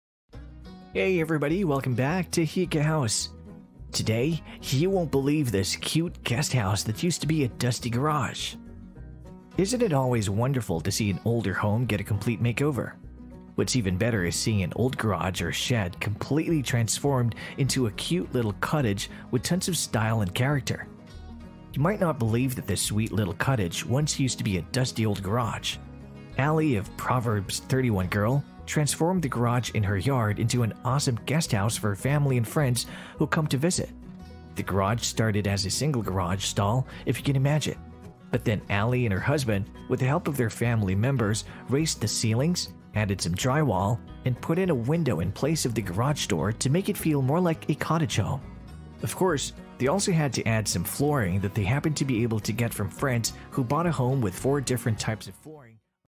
You Wont Believe This Cute Guest House Used to Be a Dusty Garage
Isn't it always wonderful to see an older home get a complete makeover? What's even better is seeing an old garage or shed completely transformed into a cute little cottage with tons of style and character. You might not believe that this sweet little cottage once used to be a dusty old garage. Allie of Proverbs 31 Girl, transformed the garage in her yard into an awesome guest house for her family and friends who come to visit. The garage started as a single garage stall if you can imagine. But then Allie and her husband with the help of their family members raised the ceilings, added some drywall and put in a window in place of the garage door to make it feel more like a cottage home. Of course, they also had to add some flooring that they happened to be able to get from friends who bought a home with 4 different types of flooring on their main level. So Allie scored the remains of the flooring. The wood flooring looks great in the little cottage, and they also added some tile flooring at the front door which will especially come in handy when people have wet boots and shoes in the winter and spring.

She also got a cute little electric fireplace for only $40 and painted it the same white as the walls to fit in. On the walls, they also created a board and batten style of wall with some boards to give it a nice country vibe. On the exterior, they gave it a fresh coat of white paint and added some black accents for the cute window shutters and the door leading into the cottage. They also added some great little planters beneath the windows for a touch of greenery and a sweet little garden on the side of the cottage. There's a nice awning over the front door and a rocking chair close by for sitting in front of the home under the shade of the trees on the property. On the inside, the cottage is adorable and super cozy. Allie has made some changes since they first created it, and it looked great, to begin with, but it looks amazing now. She added some soft white flowing curtains on the windows as well as some art above the fireplace. Then, a larger coffee table with some chairs with white slipcovers. All of the white actually looks incredible and proves that there's no such thing as too much of one colour pallet.

They also added a bed in the space which is perfect for guests coming to spend the night. As well as a great armoire to put linens in or guests can use to store their clothing and items. The bed looks super comfortable with all white linens and lots of pillows. The windows with the soft curtains stream some beautiful light into the space making it look really dreamy. On one side, she decided to put a nightstand and on the other, instead of another nightstand, she added a small desk with a chair which would be great for writing or doing some work. That's a great idea for a smaller space too, if you need a desk space but don't have enough room, just opt to change out one table for a desk instead. It also shows the amazing things you can do with a small space, so if you have a shed of your own or a small garage like this one, you could turn it into a sweet space on your property. It could also make for a great hang out spot when you need some relaxation.